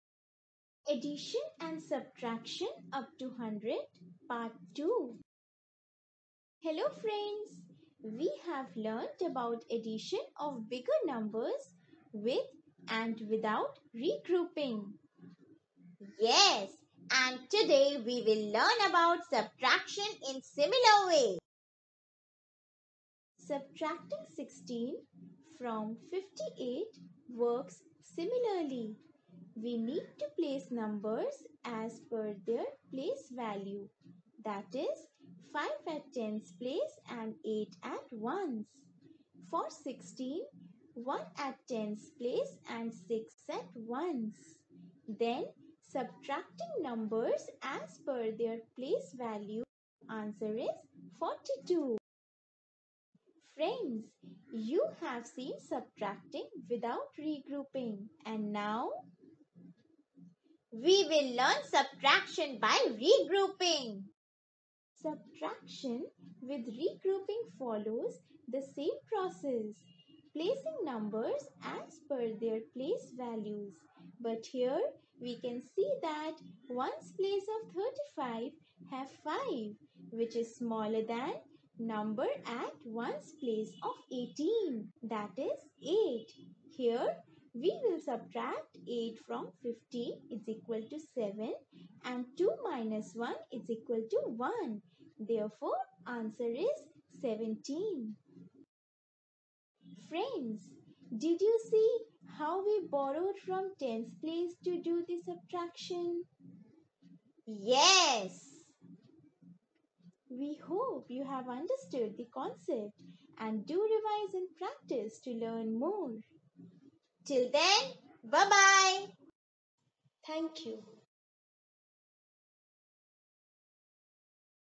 Subtraction for Numbers upto 100 l Concept of Subtraction l CBSE l NCERT l Class 1
Learn about the concept of subtraction of bigger numbers upto 100. Learn with Gia and Dodo
VafleCone is a one-stop platform for your child to learn with interactive videos, downloadable worksheets and online questionnaires.